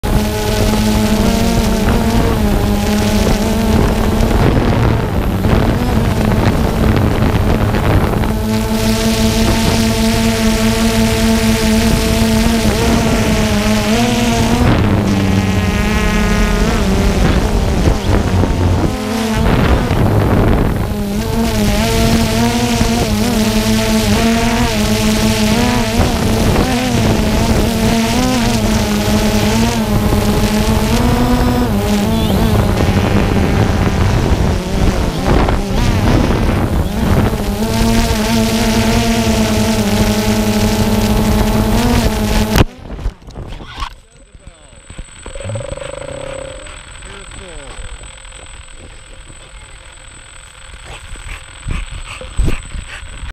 My Bulldog takes my drone out of midair!
Almost fail at 0:09. End result occurs at 0:42

I was fortunate to just loose the lcd screen on the camera. She also dented the metal face a little with her teeth. Phantom 2 and Zenmuse escaped without a mark.

Jezebel felt so proud to take that out of the air. I feel like this is how she would act if she ever got one of those squirrels she always chases..